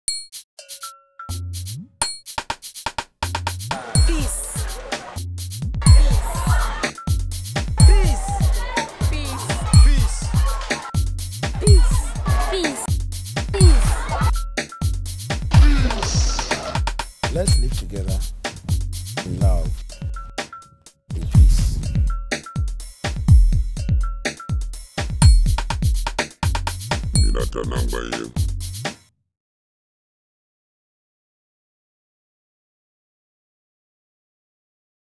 Peace 2008 Street Talk version
A short television spot we produced in close co-operation with CET-NET NGO and the National Peace Council. This is the street talk version. Anyone and everyone!